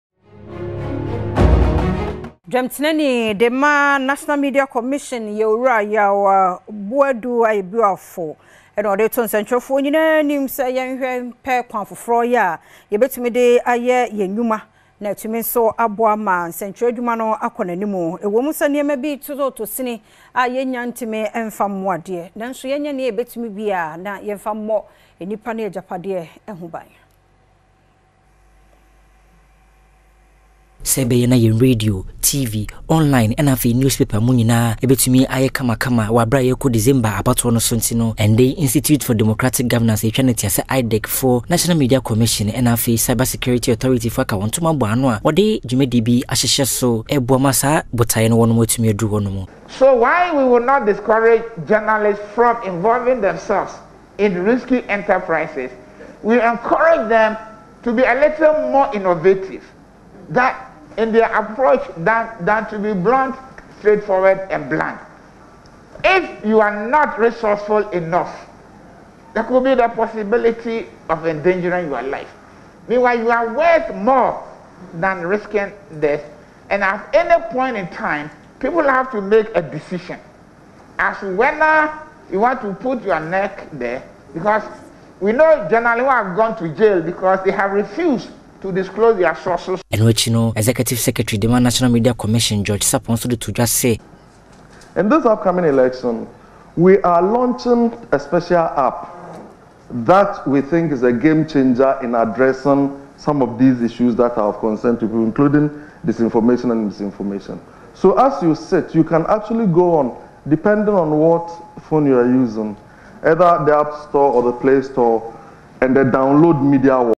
MMC chairman calls for innovative reporting to protect journalists' safety -(31-10-24
MMC chairman calls for innovative reporting to protect journalists' safety  - Premtobre Kasee (31-10-24